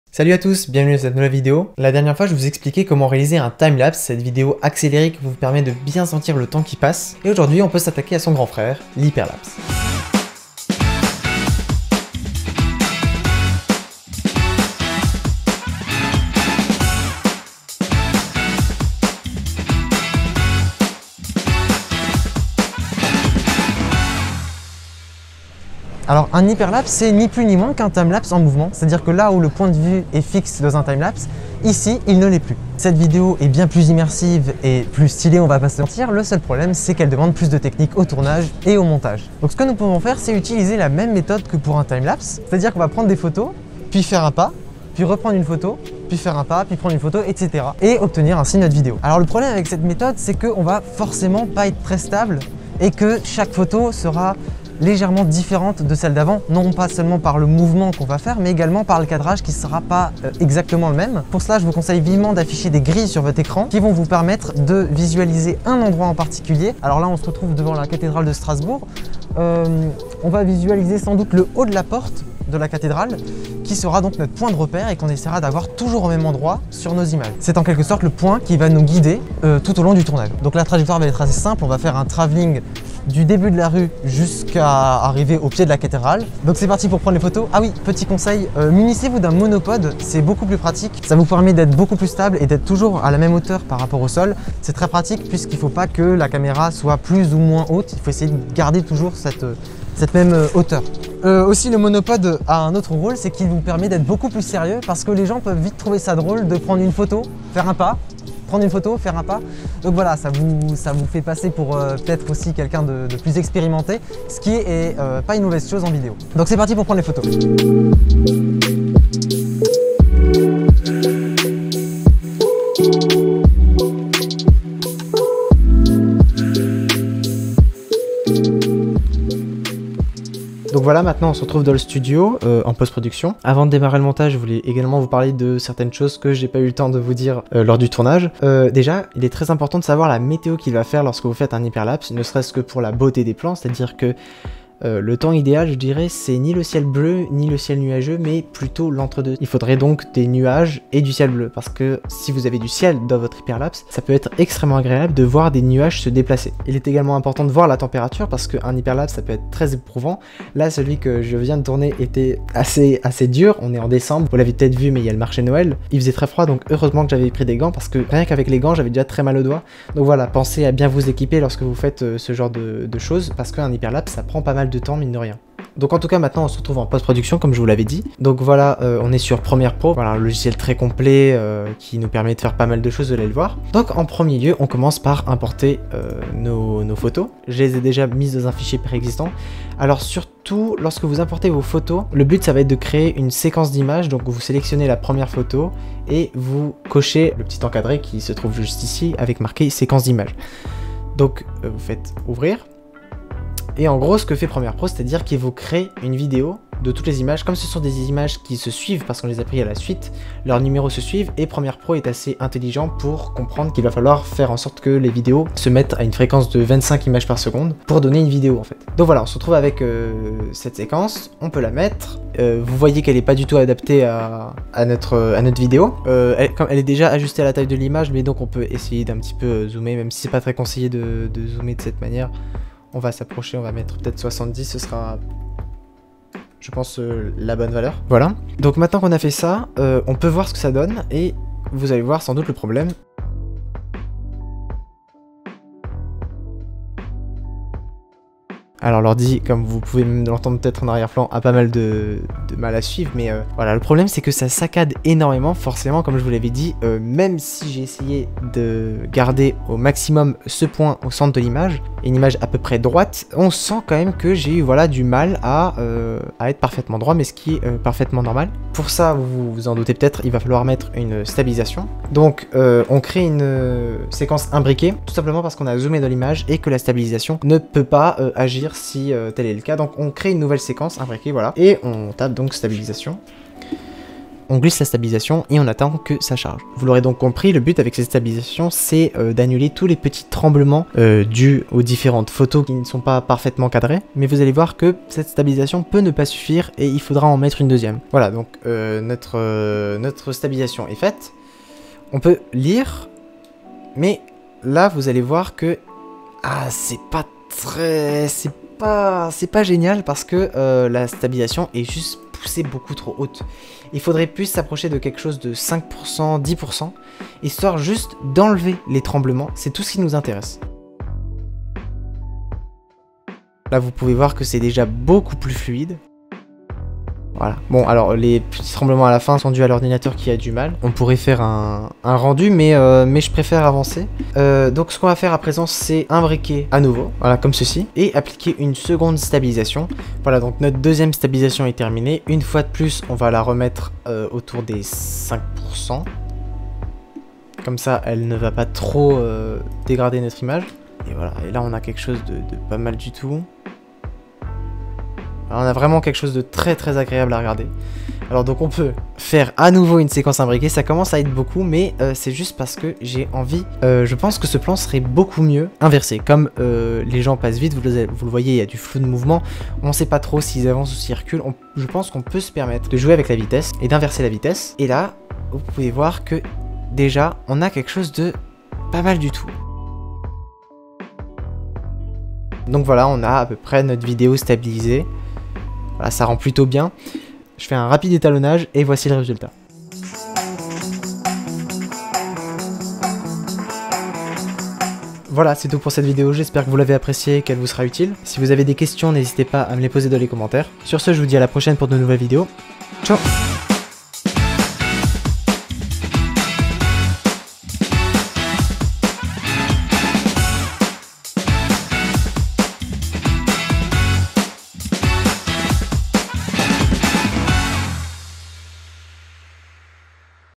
Comment faire un HYPERLAPSE ?
Après avoir vu comment faire un time lapse, voici une petite vidéo sur l'hyperlapse ! J'espère que cette vidéo vous a plu ! Si vous avez des questions, les commentaires sont faits pour ;) Alors n'hésitez pas.

Chapitres :
0:00 Intro
0:25 Qu'est ce qu'un hyperlapse et comment le tourner ?
1:57 Tournage
2:10 Montage
6:16 Résultat final
6:35 Outro